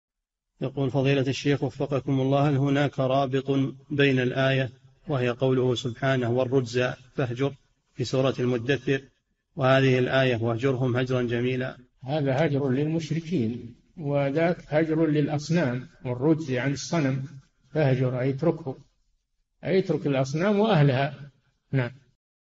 [2975- 4717] هل هناك رابط بين قوله تعالى:﴿والرجز فاهجر﴾ وقوله:﴿واهجرهم هجرا جميلا﴾؟ - صالح الفوزان
[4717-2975] هل هناك رابط بين الآية وهي قوله سبحانه: ﴿وَالرُّجْزَ فَاهْجُرْ﴾ [المدثر: 5] وهذه الآية: ﴿ وَاهْجُرْهُمْ هَجْرًا جَمِيلًا﴾ [المزمل: 10]؟
الجَوَابُ: هذا هجر للمشركين وذاك هجر للأصنام، والرُّجز يعني الصنم، فاهجره أي اتركه، أي اترك الأصنام وأهلها.
السيرة الذاتية :
للاشتراك في قناة الشيخ صالح الفوزان
لمشاهدة حلقات فتاوى على الهواء كاملة

للاستفسار والإقتراحات والدعم
رئيس الفريق
لمشروع كبار العلماء